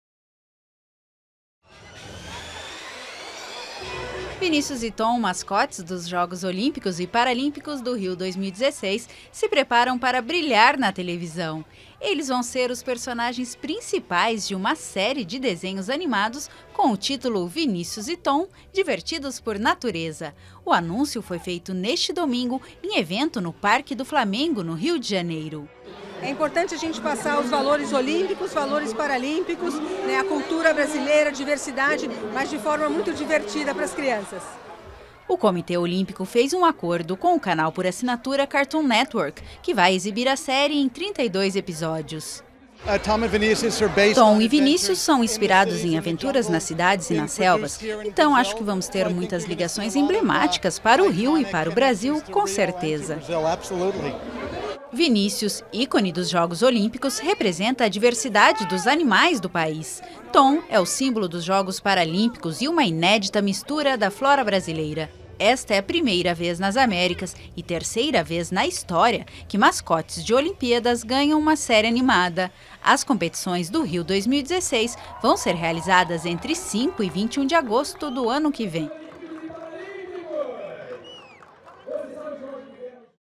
Mascotes do Rio 2016 vão virar desenho no Cartoon Network
Antes mesmo do início dos Jogos Olímpicos de 2016, os mascotes Vinícius e o Tom têm uma série de aventura pela frente. Os representantes da flora e fauna brasileiras vão ser personagens de desenho animado no canal por assinatura Cartoon Network.